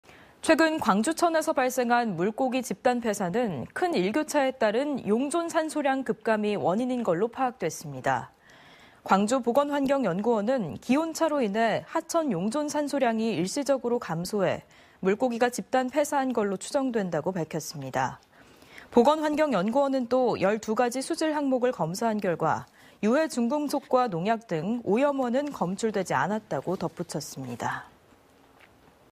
광주천 물고기 집단폐사.."큰 일교차에 용존산소량 급감"
최근 광주천에서 발생한 물고기 집단폐사는
큰 일교차에 따른 용존산소량 급감이
원인인 것으로 파악됐습니다.

광주보건환경연구원은 "기온차로 인해
하천 용존산소량이 일시적으로 감소해
물고기가 집단 폐사한것으로 추정된다"고 밝혔습니다.

보건환경연구원은 또 "12가지 수질 항목을
검사한 결과 유해 중금속과 농약 등
오염원은 검출되지 않았다"고 덧붙였습니다.